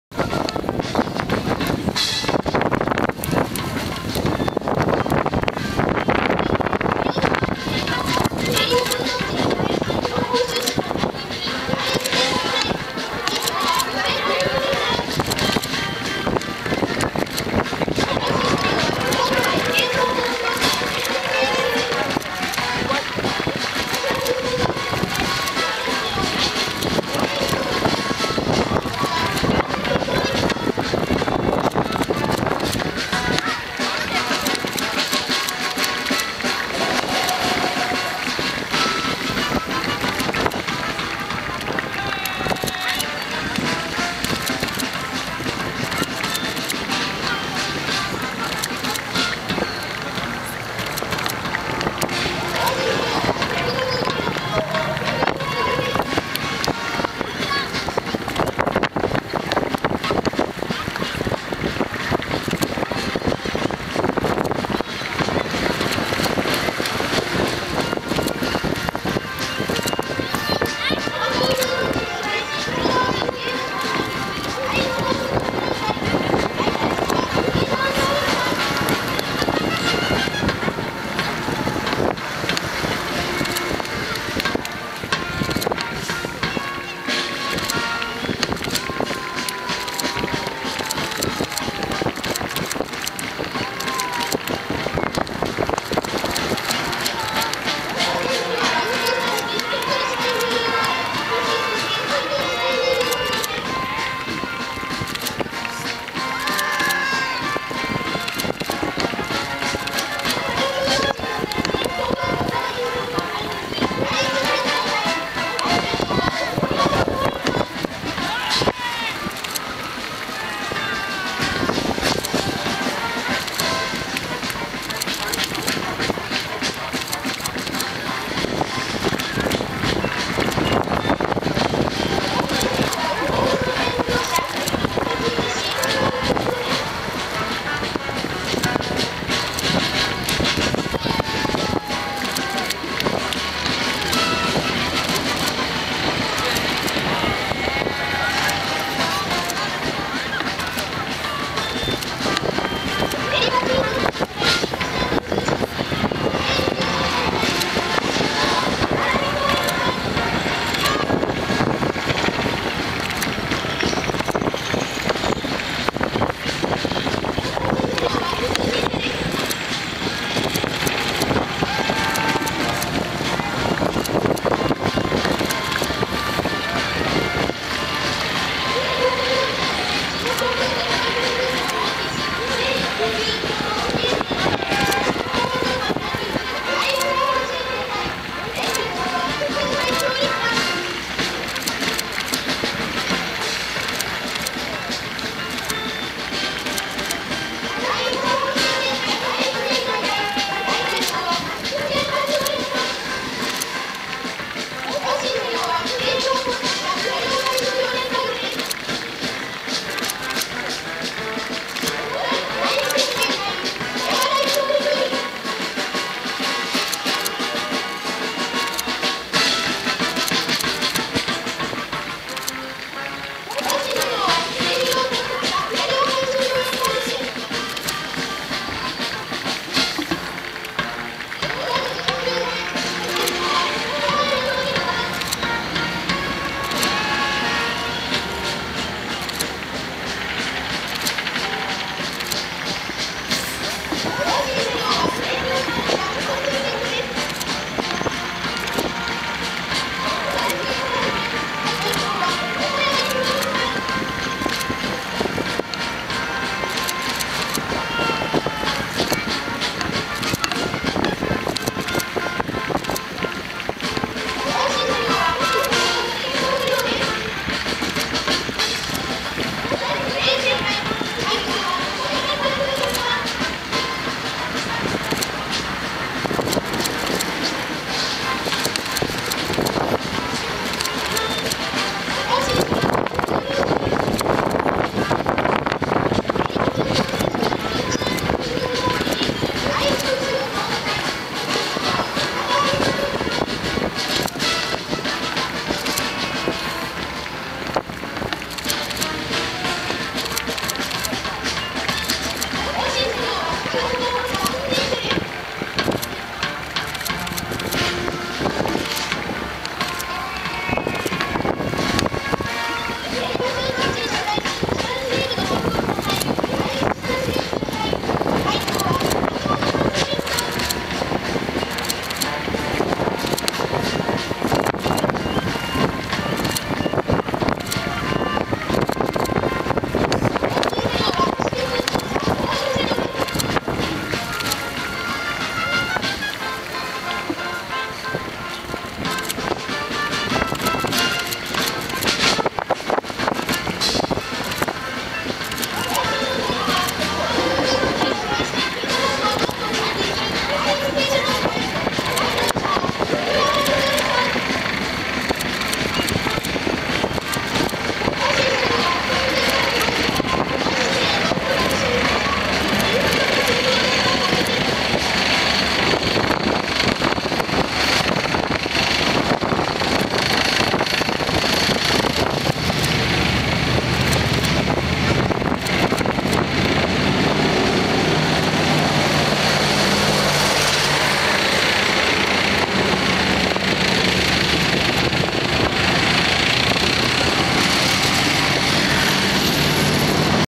車両行進
2013年4月14日、練馬駐屯地で行われた記念式典の模様です